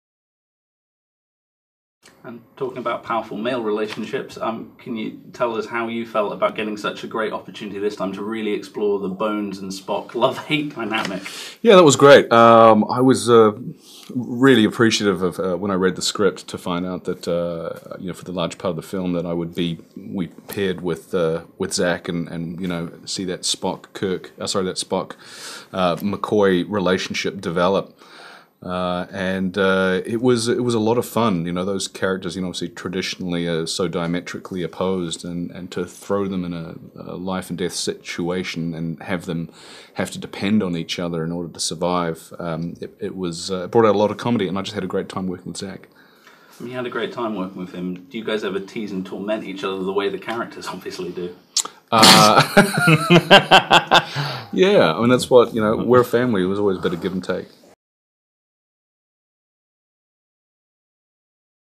Star Trek Beyond Karl Urban Interview Bones vs Spock
Red Carpet News sits down with the cast of Star Trek Beyond including Chris Pine, Zachary Quinto, Karl Urban and John Cho.  The film finds the crew of the USS Enterprise facing their most deadly battle yet at the edge of the Federations frontier.